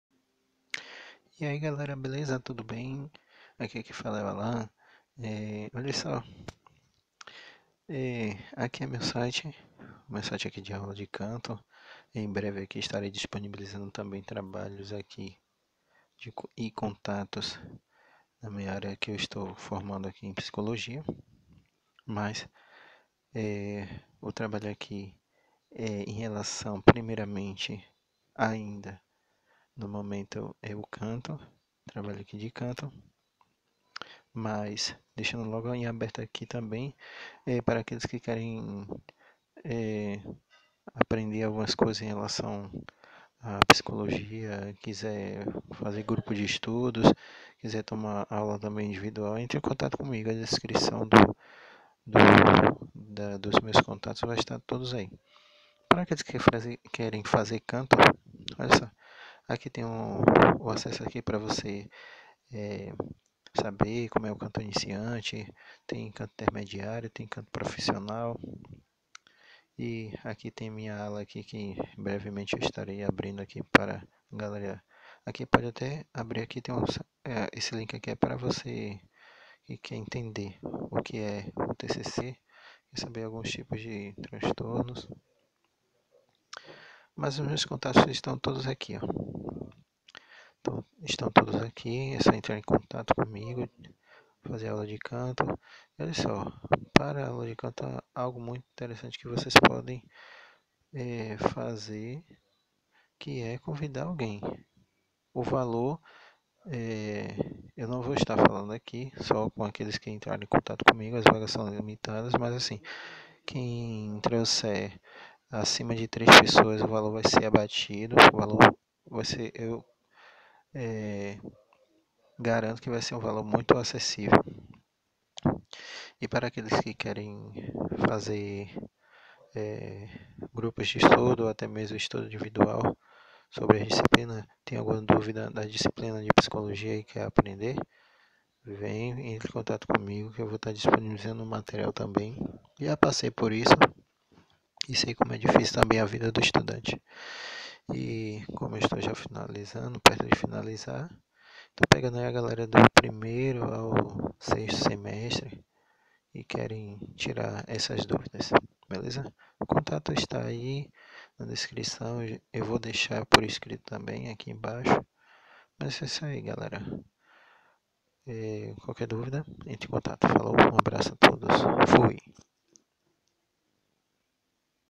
Anúncios - Aulas de Canto e Psicologia
Galerinha aula a partir do valor de R$ 80,00 reais caso você convide 3 pessoas para a aula você irá pagar apenas R$ 45,00 reais é isso mesmo.

Vagas limitadas!

Para estudantes de Psicologia do 1 ao 6 semestre somente viu galerinha, que quer aprender mais um pouco sobre os diversos assuntos via skype. Entrem em contato o site dispõe dos meus contatos.